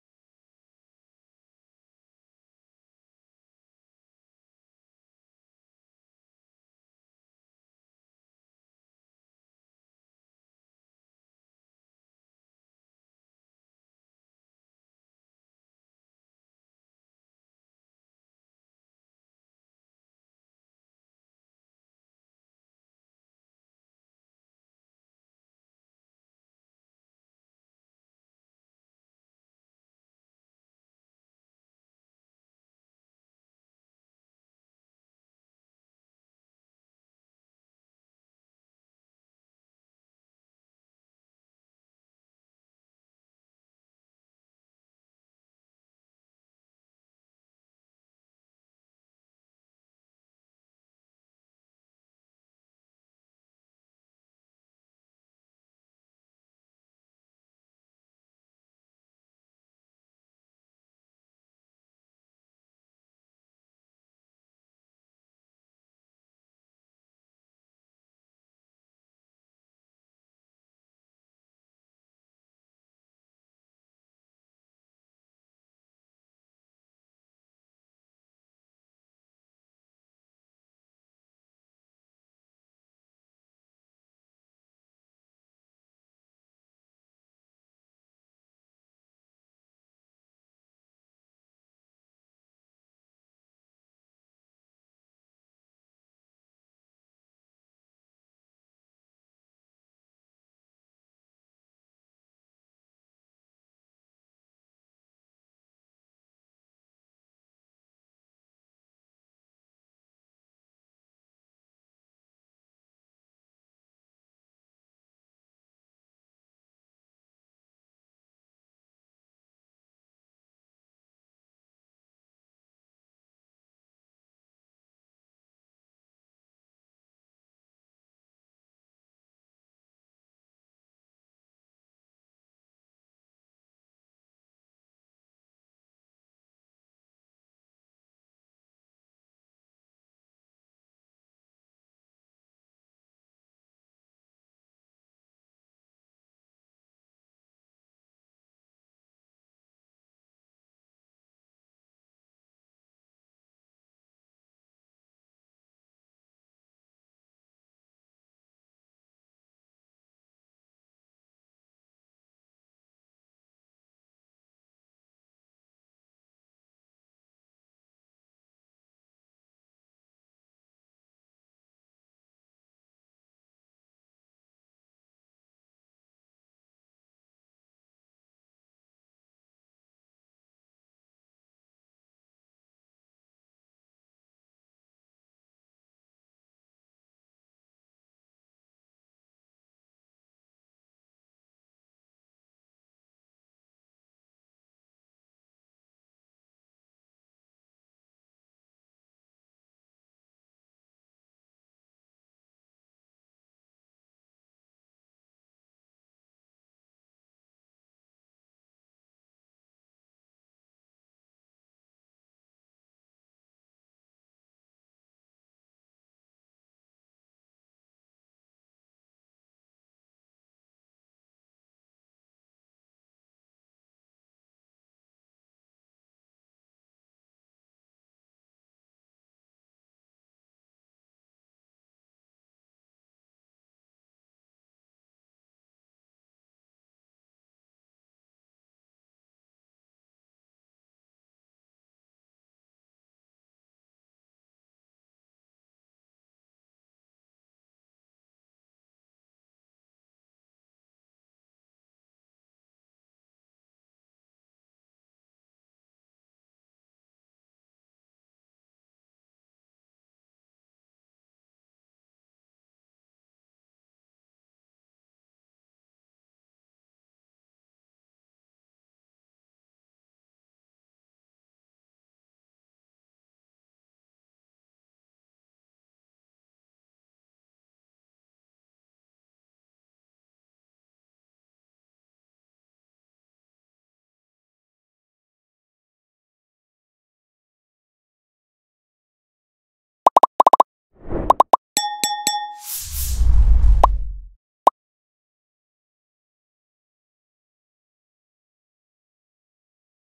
Apply the Glow Light Effect in Photoshop tutorial to get the Glow Effect in 5 minutes
In this video, you will learn about Glow Light Effect Photoshop Tricks You Should Know! In this Advanced Add Glowing Light Effect to Anything in Photoshop Tutorial, you will learn how to add glow light effects in Photoshop. Use this simple technique to add a glowing light effect to any transparent object in Photoshop. I'll show you step-by-step how to easily make a glowing light effect with superficial layers in Photoshop.